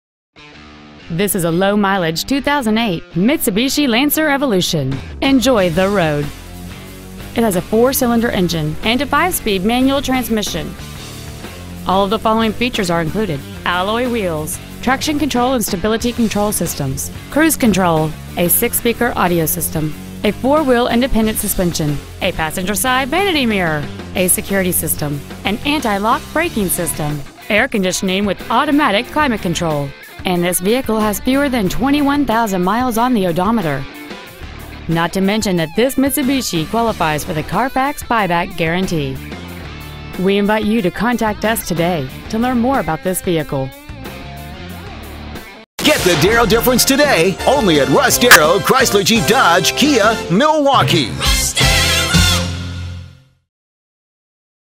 Preowned 2008 Mitsubishi Lancer Milwaukee WI
Year : 2008

 Make : Mitsubishi

 Model : Lancer

 Engine : 2

 Trans . : 5-Speed Manual

 Exterior : Rally Red Metallic

 Miles : 20,200

 Interior : Black

 Stock : J11278A

 Russ Darrow Chrysler Jeep Dodge Milwaukee

 7676 N. 76th St.